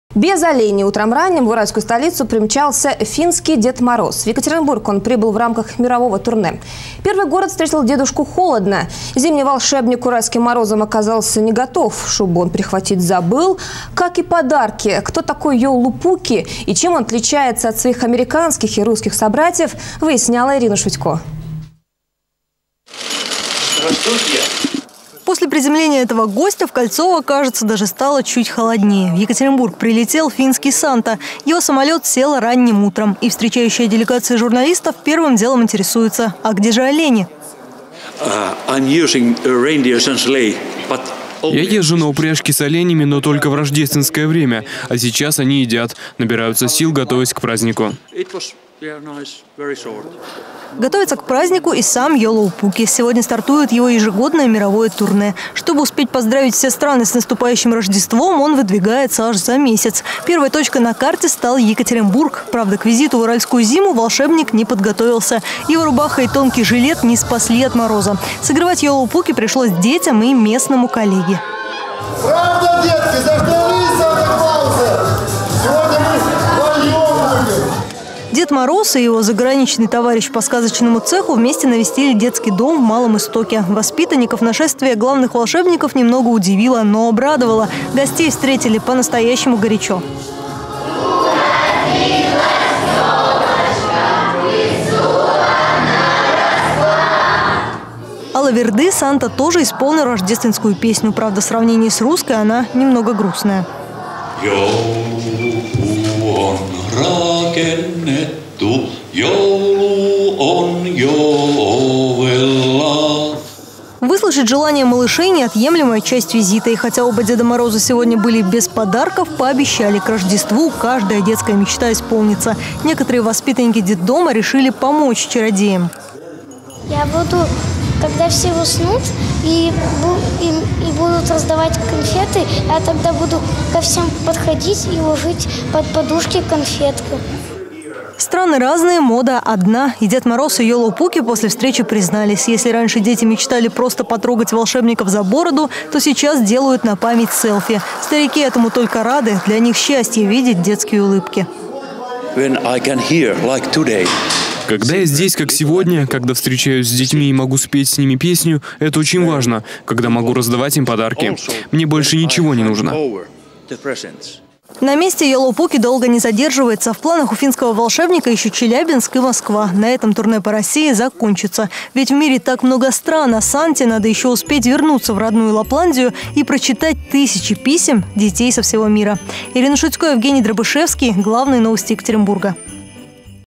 Финский Дед Мороз в Екатеринбурге
Променял оленью упряжку на бизнес-класс в самолете: в Екатеринбург прилетел Йоулупукки

В Екатеринбург финский Дед Мороз прибыл в рамках мирового турне

Кажется, после приземления этого гостя в "Кольцово" даже стало чуть холоднее. В Екатеринбург прилетел финский Санта. Его самолет сел ранним утром. И встречающая делегация журналистов первым делом интересуется: а где же олени? Готовится к празднику и сам Йоулупукки. Сегодня стартует  его ежегодное мировое турне. Чтобы успеть поздравить все страны с наступающим Рождеством, он выдвигается аж за месяц. Первой точкой на карте стал Екатеринбург. Правда, к визиту в уральскую зиму волшебник не подготовился - его рубаха и тонкий жилет не спасли от мороза. Согревать Йоулупукки пришлось детям и местному коллеге. Дед Мороз и его заграничный товарищ по сказочному цеху вместе навестили детский дом в Малом Истоке. Воспитанником нашествие главных волшебников немного удивило, но обрадовало - гостей встретили по-настоящему горячо. Алаверды Санта тоже исполнил Рождественскую песню. Правда, в сравнении с русской, она немного грустная. Выслушать желания малышей - неотъемлемая часть визита. И хотя оба Деда Мороза сегодня были без подарков, пообещали - к Рождеству каждая детская мечта исполнится. Некоторые воспитанники детдома решили помочь чародеям. Страны разные - мода одна. И дед мороз и Йоулупукки после встречи признались, если раньше дети мечтали просто потрогать волшебников за бороду, то сейчас делают на память селфи. Старики этому только рады - для них счастье видеть детские улыбки. На месте Йоулупукки долго не задерживается - в планах у финского волшебника еще Челябинск и Москва - на этом турне по России закончится. Ведь в мире так много стран, а Санте надо еще успеть вернуться в родную Лапландию и прочитать тысячи писем детей со всего мира.